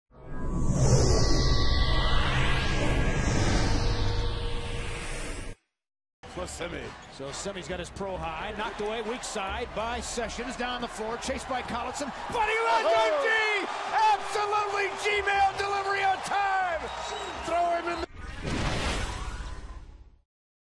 Steal of the Night 2/15/2012
Kyrie Irving comes up with the steal and gets it ahead to Ramon Sessions and he toss the alley-oops to Alonzo Gee.